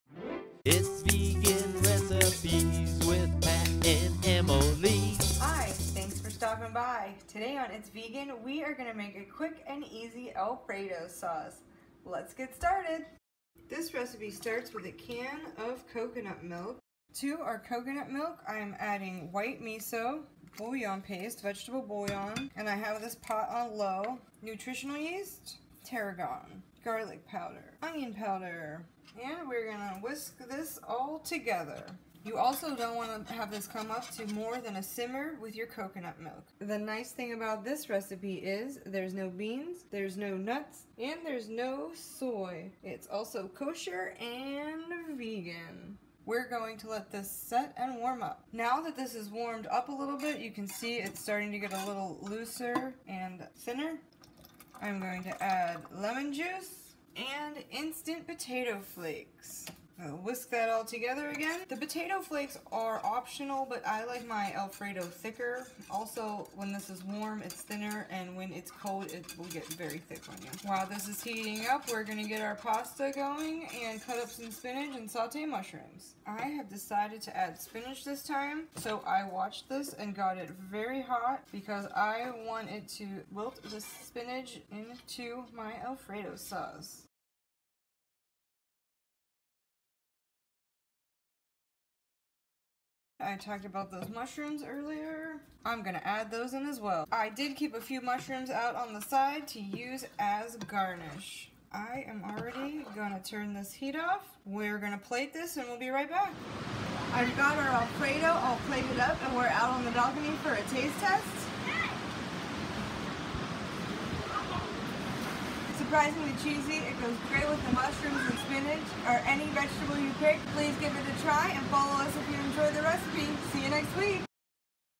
Vegan Alfredo with Spinach and Mushrooms
This is one of the best vegan Alfred sauces around. Creamy and flavorful.  The recipe makes 3-4 servings.

1 can of coconut milk
1-2 teaspoons Miso
2 tablespoons nutritional yeast
2 teaspoon vegan bouillon paste
2 tablespoons lemon juice
2 tablespoons tarragon
2 tablespoon garlic powder
4 teaspoon potato flakes
1 small bunch of fresh spinach-rough chop
2 cups sliced mushrooms
6-8 ounces of pasta such as fettuccini or linguine
Cook pasta according to package instructions. Drain and keep warm. Set aside.
Sauté the mushrooms, set them aside, and keep warm.
Pour coconut milk into a medium saucepan. Heat until warm, then stir in miso, nutritional yeast, bouillon, lemon juice, tarragon, garlic powder, and potato flakes. Once all ingredients are well blended and the sauce is smooth increase heat, but do not boil.  Stir in the spinach until the spinach is wilted. Set a few mushrooms aside for garnishing. Stir in mushrooms.
Place hot pasta on a serving plate, and top with the Alfredo Sauce. Garnish with remaining mushrooms.